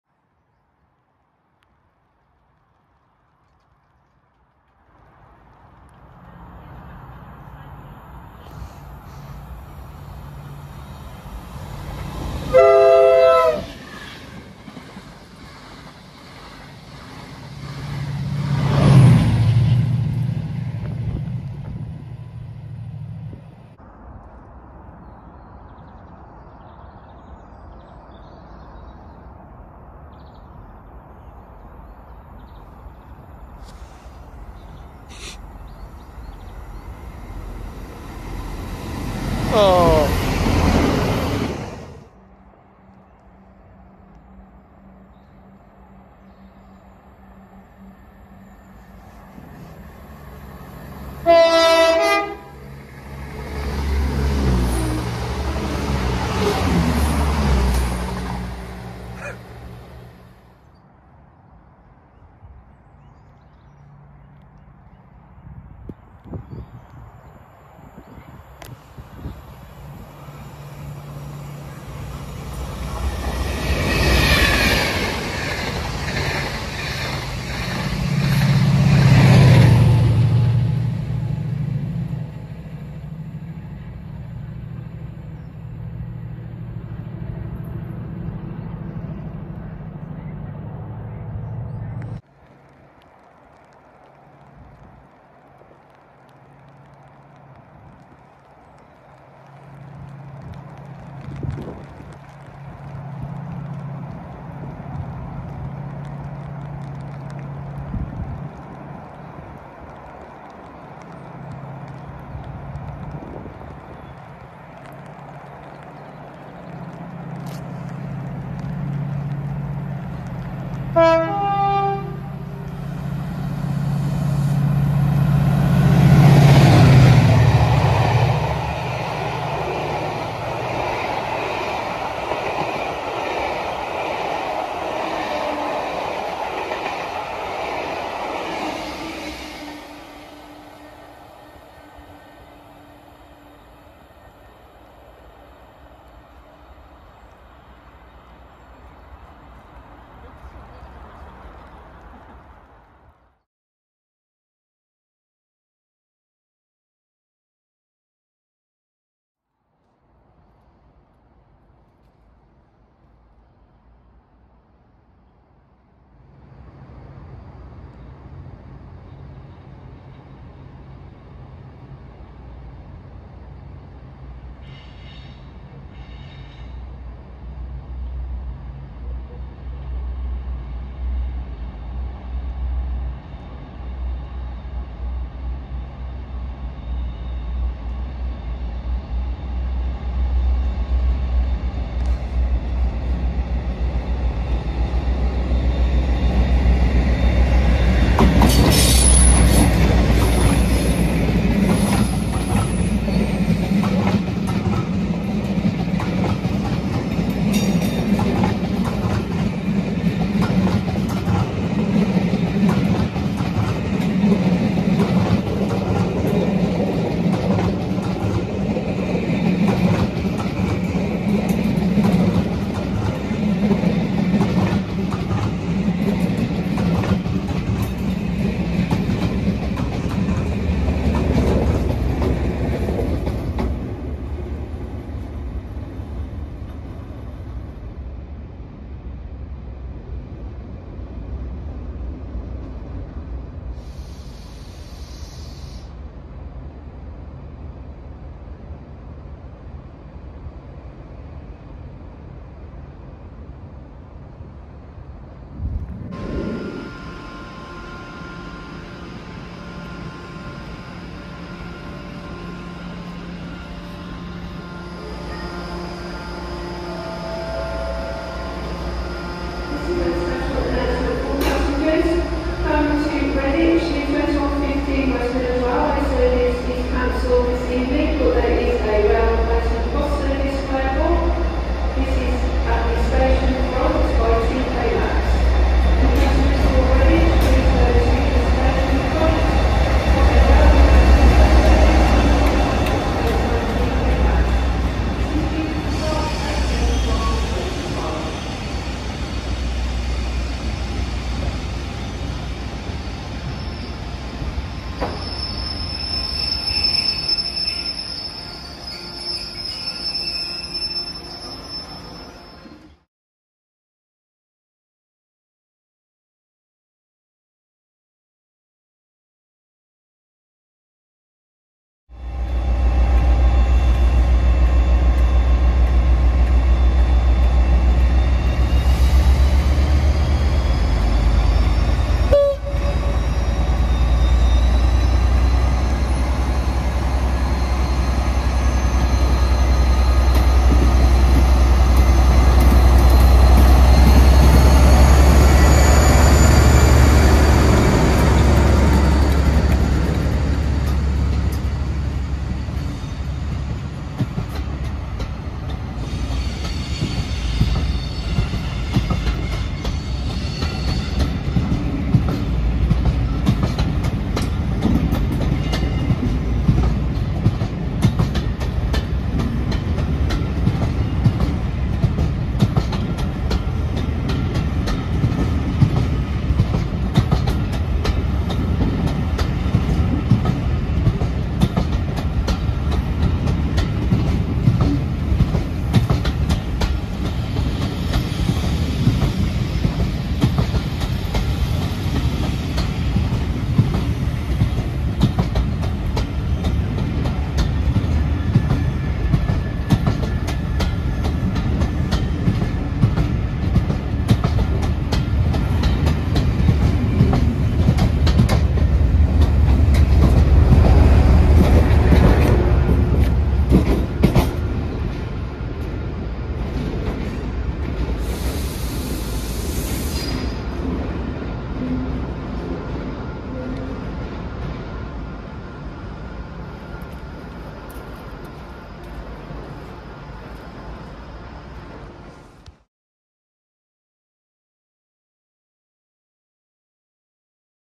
68018,66434 & 57309 on Pathfinder Link’N’Chord ‘the Blue Boys’ railtour —Various locations 19/02/22
The tour first sees me visit widney manor to see 68018 lead with 66434 on the rear, due to problems coming out of oxford the tour was delayed by just over 2 hours, so i decided to film some non stoppers while i waited. The weather got worse by the time it came through.
After going home for a while i went back out and headed for walsall to catch the return journey to didcot where 57309 joined the train, i then chased it to new street to see it arrive in and depart.
Not a bad day out either
And spent chasing it with 5083 steam videos own Tom